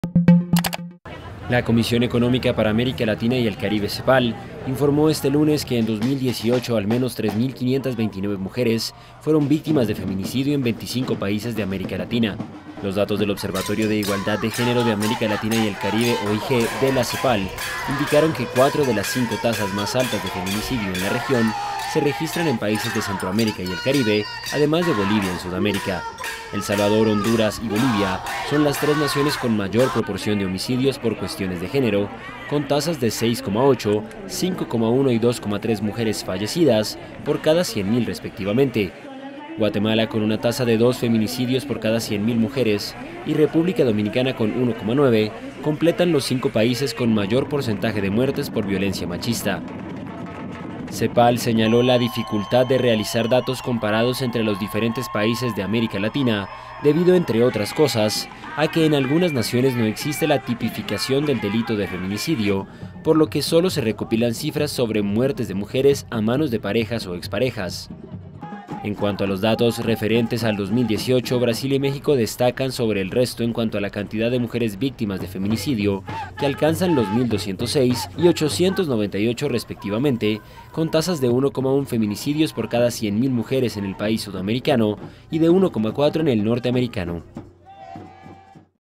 Cepal: 3.529 mujeres fueron víctimas de feminicidio en América Latina en 2018
La Comisión Económica para América Latina y el Caribe (Cepal), informó este lunes de que en 2018 al menos 3.529 mujeres fueron víctimas de feminicidio en 25 países de América Latina.